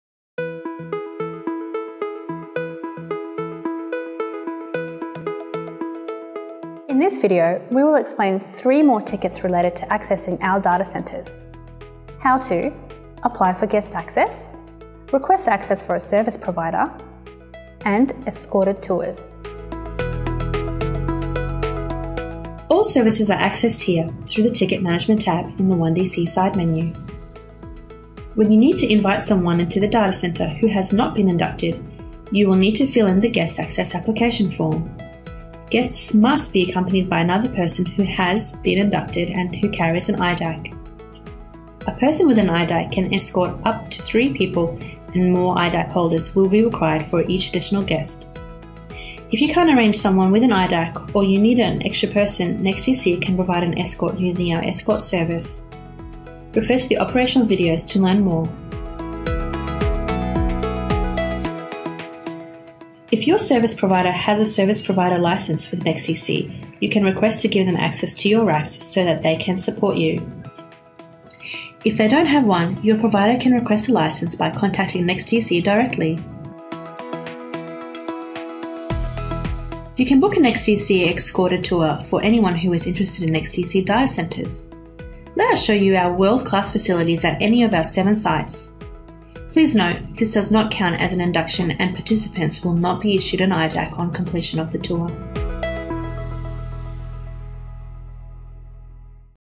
ONEDC Ticket Management - Apply for guest access, service provider access and escorted tours
High level information and examples of submitting requests to get access to a NEXTDC data centre.
Guest access: 0:28
Service provider: 1:00
Escorted tour: 1:17